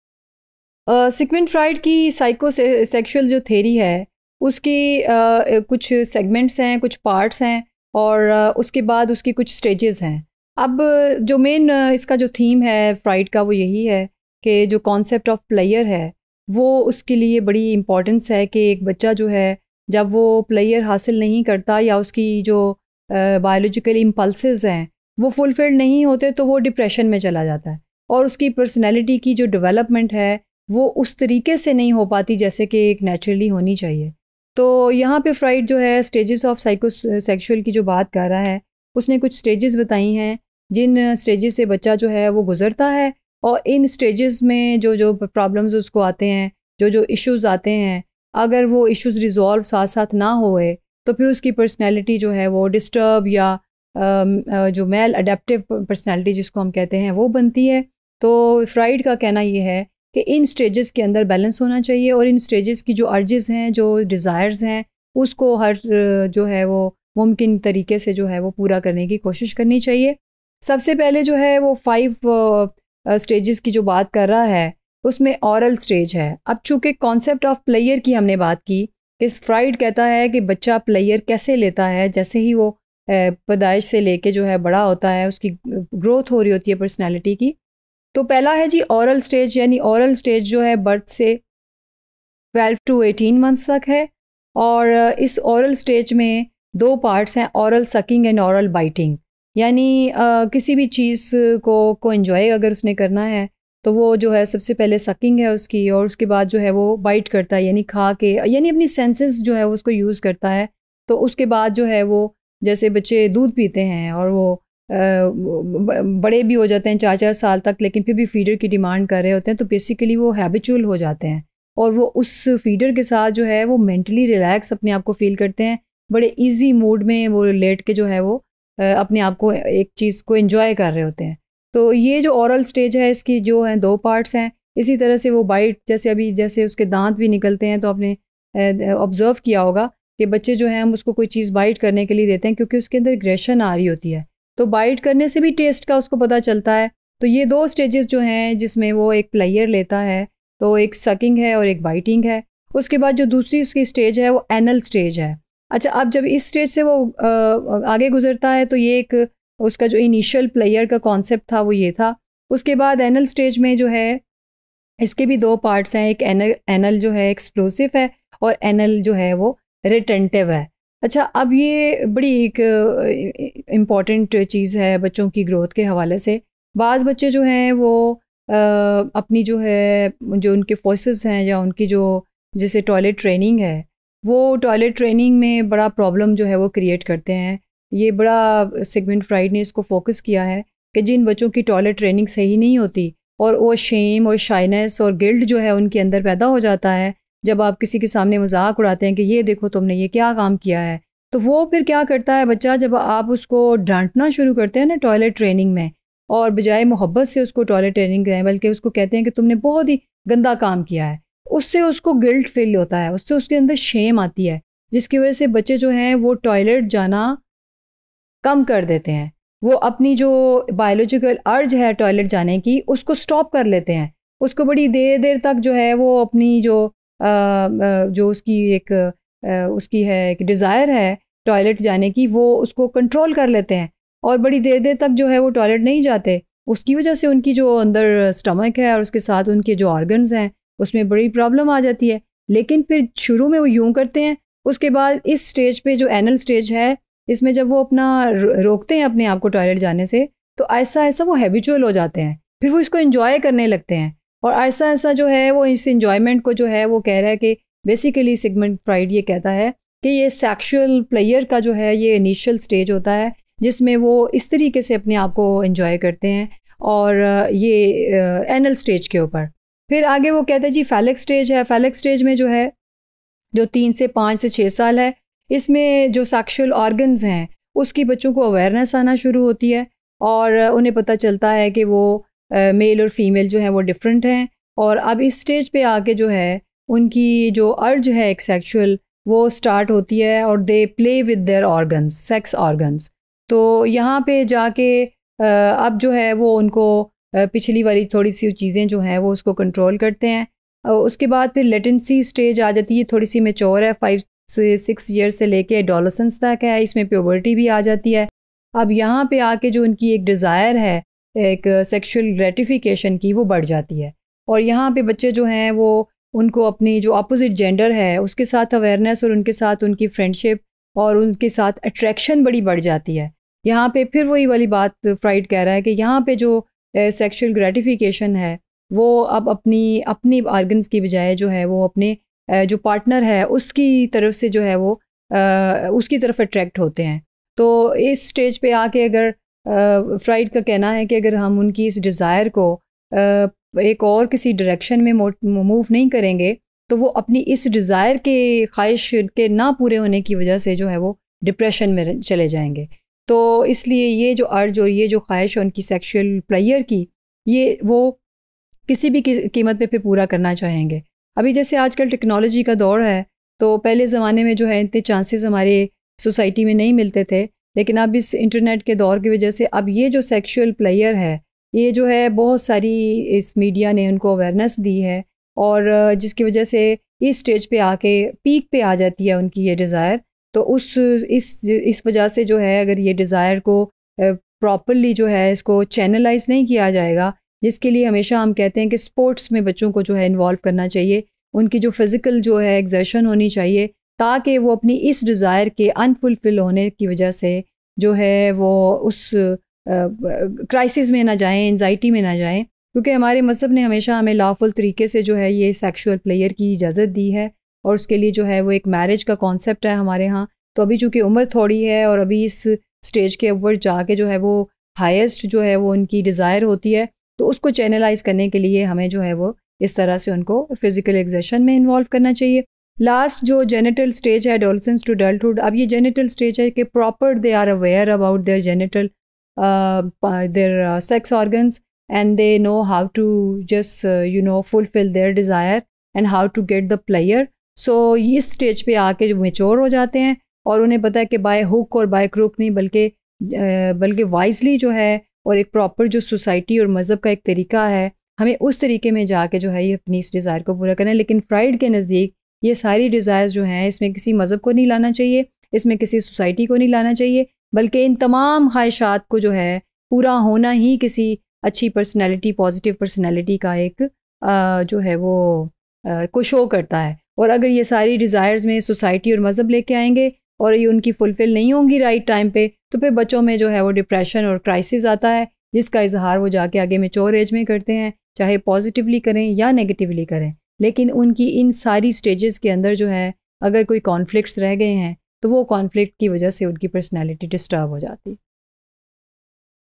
Stages of Psychosexual Theory | Human Development and Learning | EDU302_Topic039
Topic039 - Stages of Psychosexual Theory
By Dr. Sumaira Rashid